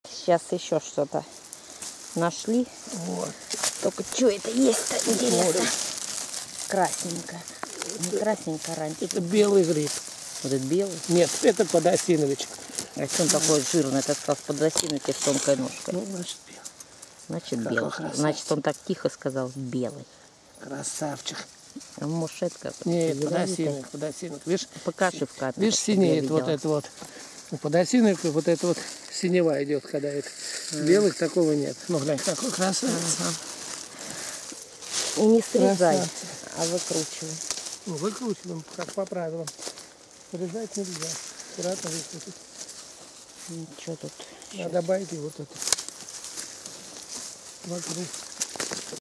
Воронеж, собираем грибы, подосиновики!
Пришли в лес за грибами!